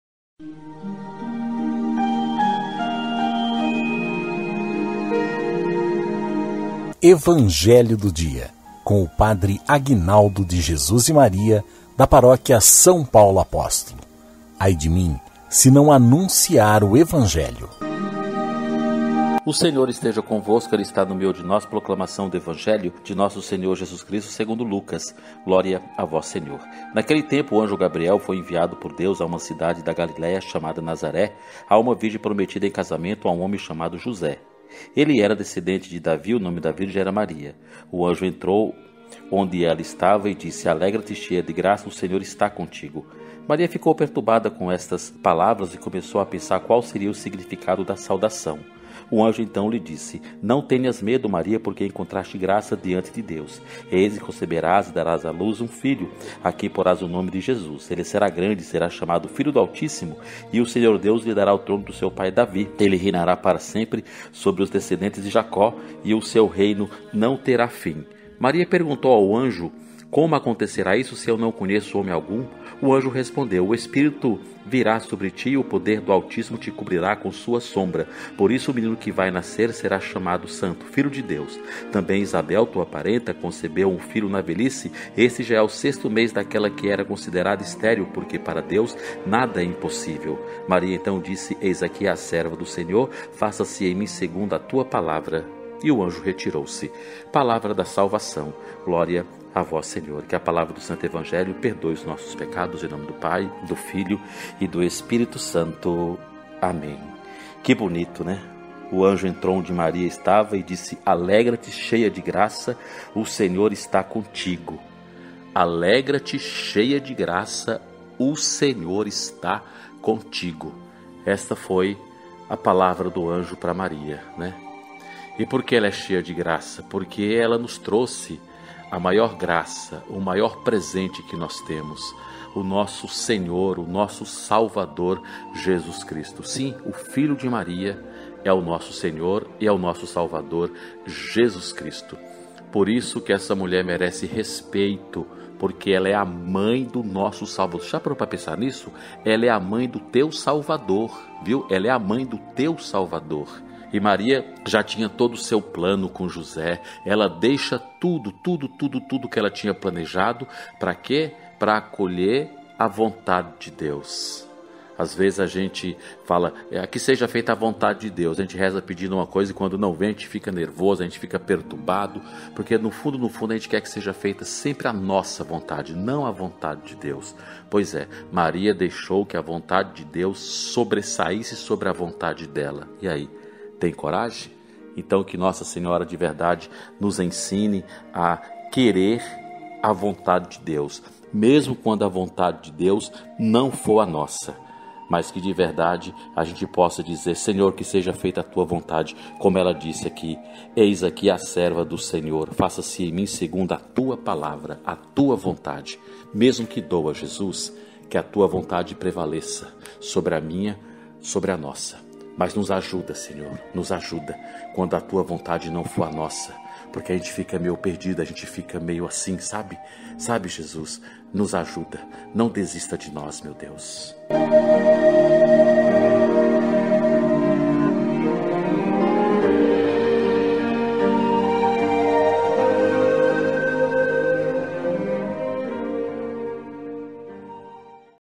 Evangelho do dia 22 de Agosto de 2024 (Lucas 1, 26-38)
Evangelho do dia 22 de Agosto de 2024 (Lucas 1, 26-38) Com o Padre Agnaldo de Jesus e Maria da Paróquia São Paulo Apóstolo - Diocese de Piracicaba.